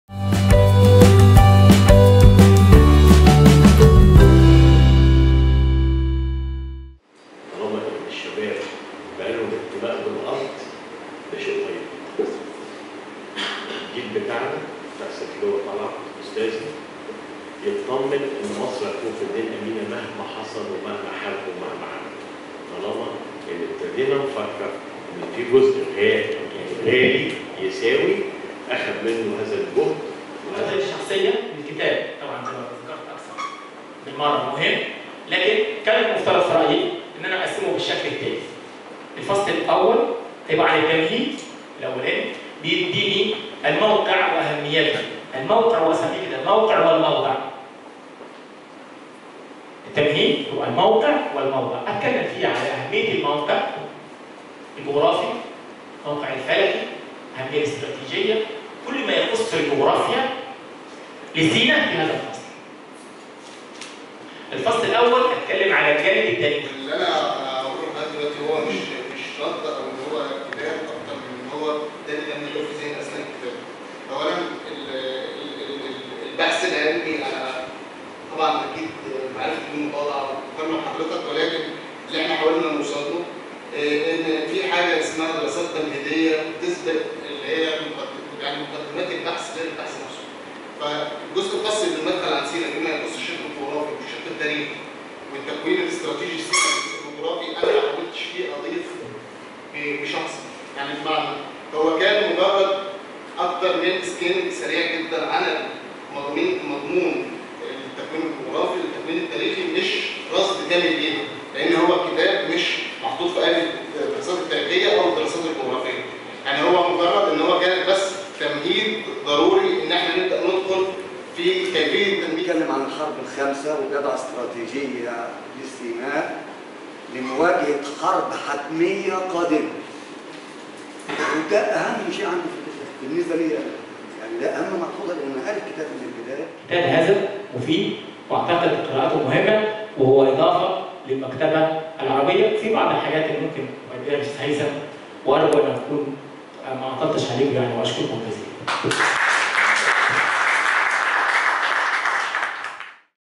ندوة ثقافية وحفل توقيع لكتاب «سيناء.. استراتيجية الدفاع والتنمية»
أقامت مكتبة القاهرة ندوة ثقافية وحفل توقيع 6 مساء الأحد 8 نوفمبر  لمناقشة كتاب «سيناء إستراتيجية الدفاع بالتنمية» الصادر عن دار البديل للطباعة والنشر والتوزيع.
تحدث فى اللقاء مؤلف الكتاب الكاتب «هيثم البشلاوى» والدكتور «راضى محمد جودة» عضو الجمعية المصرية التاريخية وقام بتقديم وإدارة اللقاء الثقافى الأستاذ «ياسر مصطفى» مدير المكتبة.

تابعوا البديل